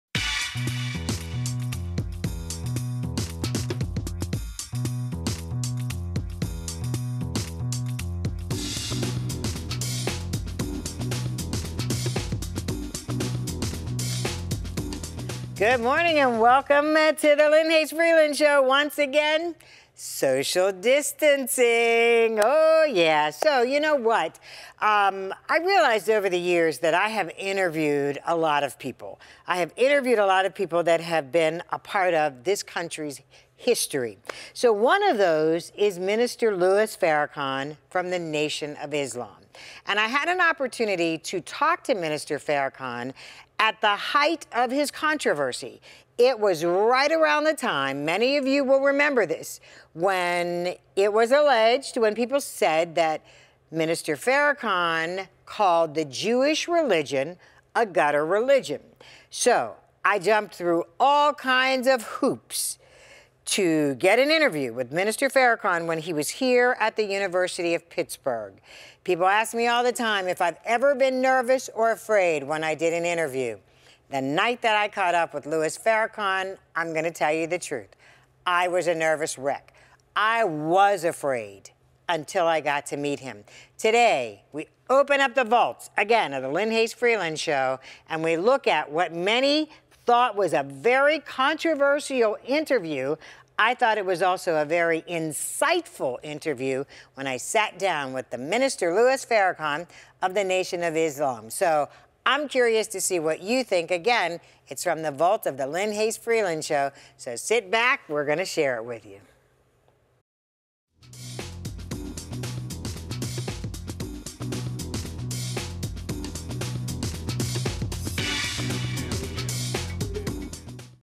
Opening The Vault: Interview With Louis Farrakhan Part 1
Lynne Hayes Freeland recalls an interview with Louis Farrakhan from back in 1994.